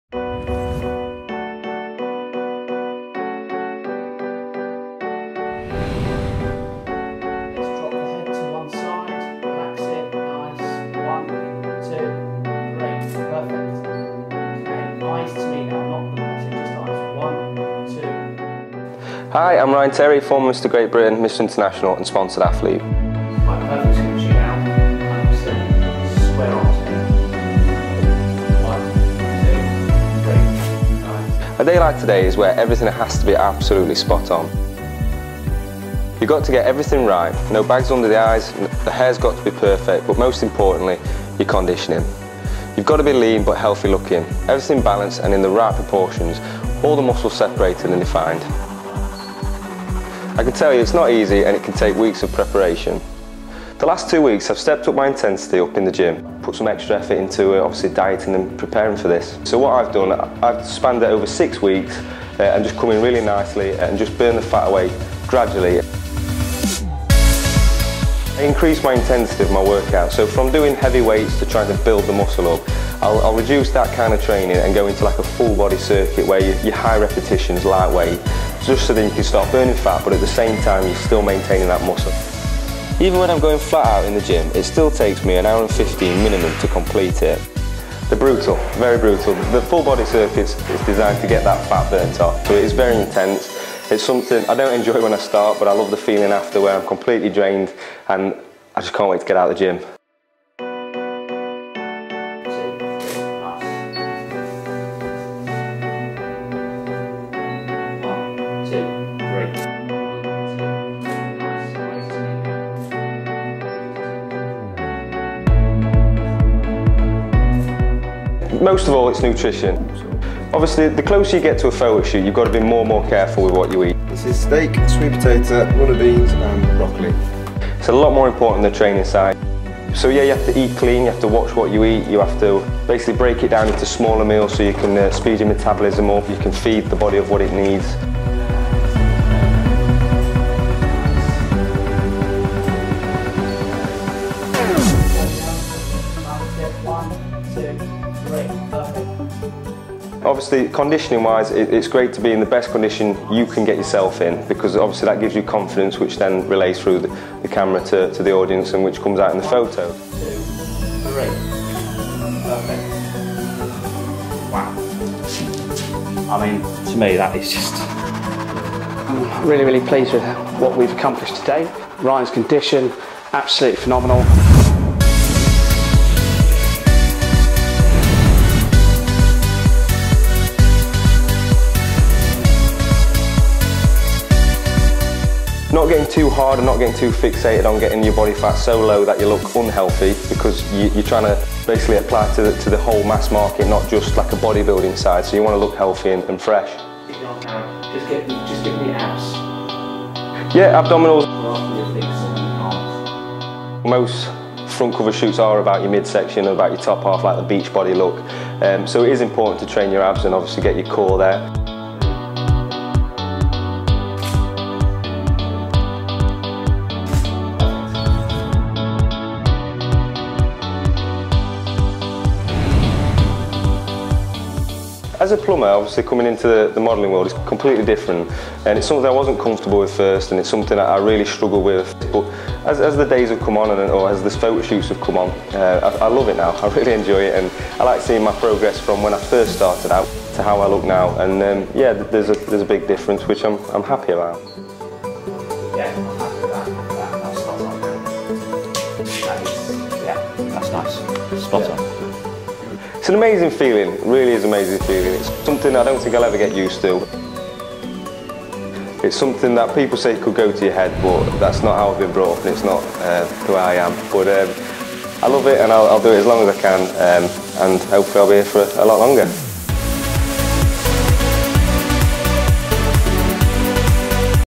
RYAN TERRY- HOW TO LOOK LIKE A COVER MODEL
To be a cover model, you have to have a great face and great body.  But even then, there's a lot more to it.  To get into cover model shape takes weeks of training and diet.  It's all about conditioning on the day - and here, one of the UK's top fitness models, Ryan Terry, takes us through the whole process. From gym training for leaning down, through to diet in the days before the shoot.